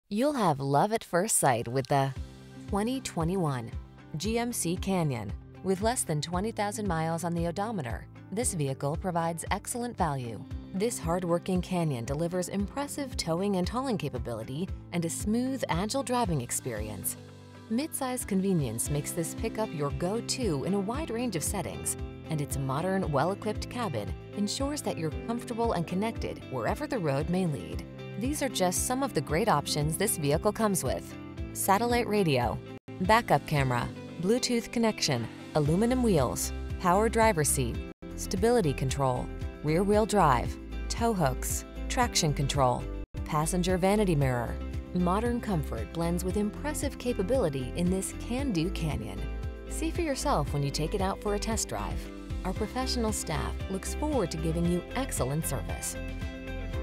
2021 GMC Canyon 2WD Elevation Standard Sequin, San Antonio, San Marcos, Austin, Schertz TX
Pre-Owned 2021 GMC Canyon 2WD Elevation Standard available at Gunn CDJR located in 1550 W Kingsbury St, Seguin, TX, 78155.
Stock#: J240196B - VIN#: 1GTH5BEA9M1211585

Servicing the Sequin, San Antonio, San Marcos, Austin, Schertz area.

Please mention the stock# J240196B when you contact the dealership for this vehicle.

You'll have love at first sight with the   2021  GMC Canyon.  This hard-working Canyon delivers impressive towing and hauling capability and a smooth, agile driving experience. Mid-size convenience makes this pickup your go-to in a wide range of settings, and its modern, well-equipped cabin ensures that you're comfortable and connected wherever the road may lead. These are just some of the great options this vehicle comes with: Satellite Radio, Back-Up Camera, Bluetooth® Connection, Aluminum Wheels, Power Driver Seat, Stability Control, Rear Wheel Drive, Tow Hooks, Traction Control, Passenger Vanity Mirror. Modern comfort blends with impressive capability in this can-do Canyon. See for yourself when you take it out for a test drive. Our professional staff looks forward to giving you excellent service!